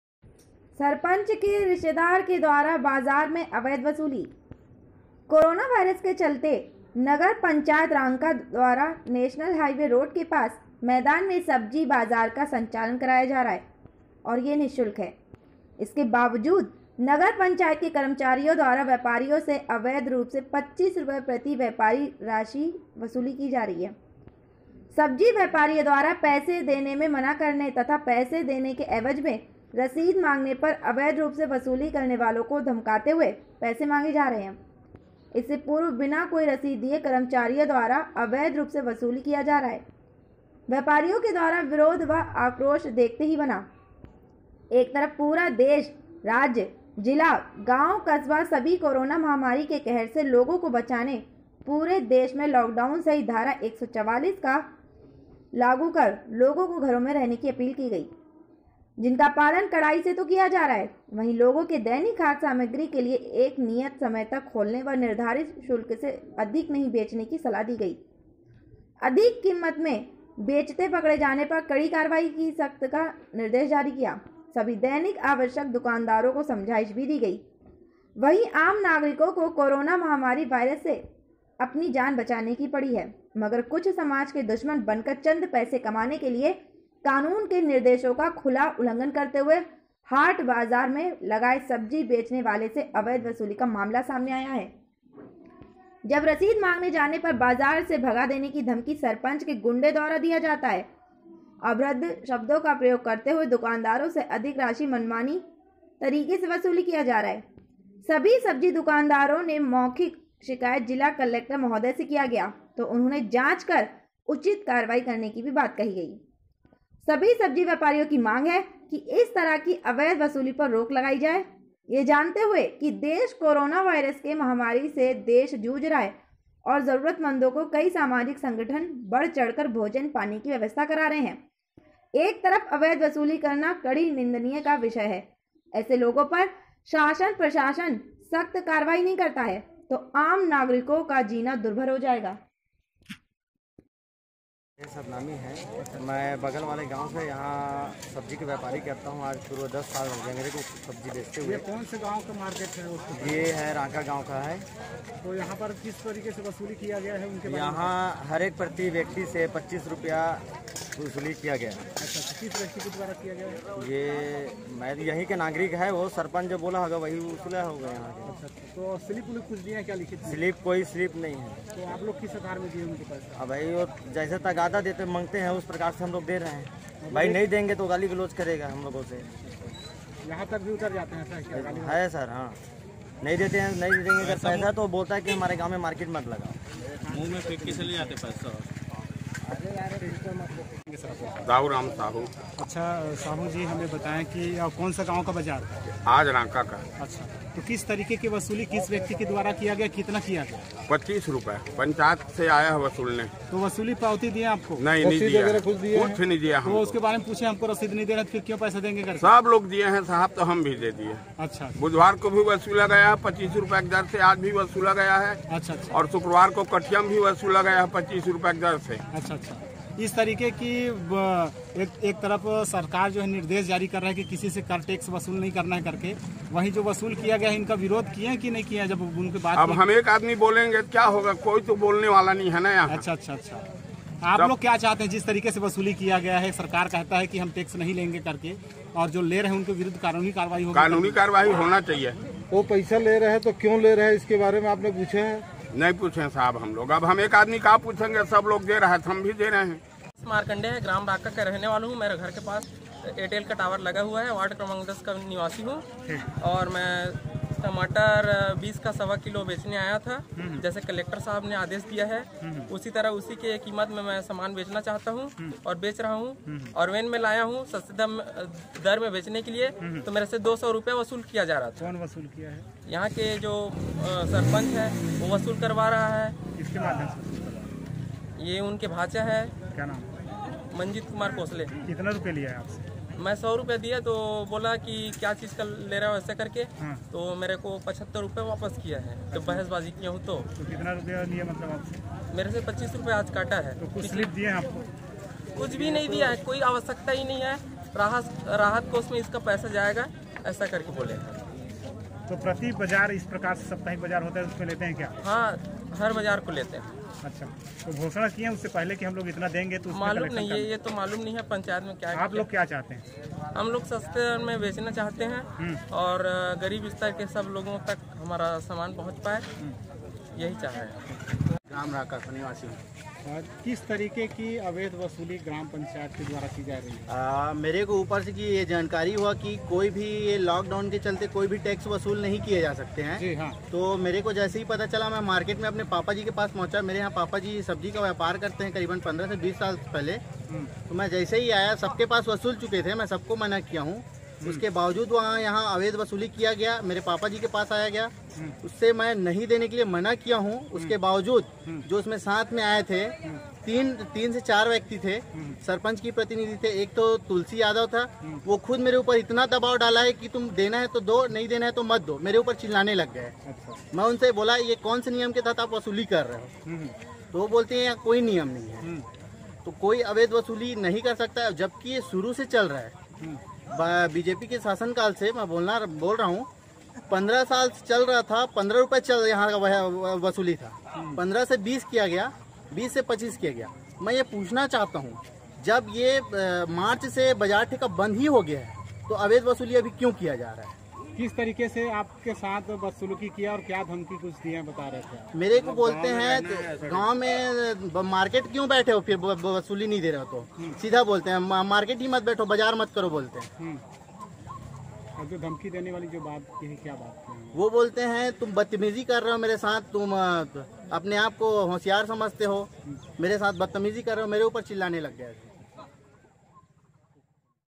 सरपंच के रिश्तेदार के द्वारा बाजार में अवैध वसूली
कोरोना वायरस के चलते नगर पंचायत रांका द्वारा नेशनल हाईवे रोड़ के पास  मैदान में सब्जी बाजार का संचालन कराया जा रहा है। और यह नि:शुल्क है। इसके बावजूद नगर पंचायत के कर्मचारियों द्वारा व्यापारियों से अवैध रूप से 25 रुपए प्रति व्यापारी राशि वसूली की जा रही।सब्जी व्यापारियों द्वारा पैसे देने मे मना करने तथा पैसे  देने के एवज मे रसीद मांगने पर अवैध रूप से वसूली करने  लोगों को धमकाते हुए पैसे मांगे जा रहे है । इससे पूर्व बिना कोई रसीद  दिये कर्मचारियों द्वारा अवैध रूप से वसूली किया जा रहा हैं। व्यापारियों के द्वारा विरोध व आक्रोश देखते  ही बना ।
 एक तरफ पूरा देश ,राज्य, जिला, गांव कस्बा सभी कोरोना महामारी के कहर से लोगों को बचाने पूरे देश में लाक डाउन सहित धारा 144 लागू कर लोगों को घरो में रहने की अपील की गई हैं जिनका पालन कड़ाई से तो किया जा रहा है ।वहीं लोगों के दैनिक खाद्य सामग्री के लिए एक नियत समय तक खोलने व निर्धारित शुल्क से अधिक नहीं बेचने की सलाह दी गई।अधिक कीमत में बेचते  पकड़े जाने पर कड़ी कार्यवाही का सख्त निर्देश जारी किया ।सभी  दैनिक आवश्यक दुकानदारों को समझाइश भी दी गई । वही आम नागरिकों को कोरोना महामारी वायरस से अपनी जान बचाने की पड़ी है, मगर कुछ समाज के दुश्मन बनकर चंद पैसे कमाने के लिए कानून के निर्देशों का खुला उल्लंघन करते हुये हाट बाजार में लगाए सब्जी बेचने वाले से अवैध वसूली का मामला सामने आया है।जब रसीद मांगने जाने पर  बाजार से भगा देने की धमकी सरपंच के गुंडे द्वारा दिया जाता है। अभद्र शब्दों का प्रयोग करते हुये दुकानदारों से अधिक राशि मनमानी तरीके से वसूली किया जा रहा है।सभी  सब्जी दुकानदारों ने मौखिक शिकायत जिला कलेक्टर महोदय से किया गया ।तो उन्होंने जांच कर उचित कार्यवाही करने की बात कही गई है ।सभी सब्जी व्यापारियों की मांग है कि इस तरह की अवैध वसूली पर रोक लगाई जाये ।यह जानते हुए कि देश कोरोना वायरस के महामारी से देश जूझ रहा है और जरूरतमंदों को कई सामाजिक संगठन बढ़-चढ़कर भोजन पानी की व्यवस्था करा रहे हैं। एक तरफ अवैध वसूली करना कड़ी निंदनीय का विषय है । ऐसे लोगों पर शासन प्रशासन सख्त कार्यवाही नहीं करता हैं, तो आम नागरिकों का जीना दुर्भर हो जाएगा ।

मारखंडे रांका निवासी

बंजारे ,सब्जी व्यापारी

अविनाश घृतलहरे

दाउ राम साहू,सब्जी व्यापारी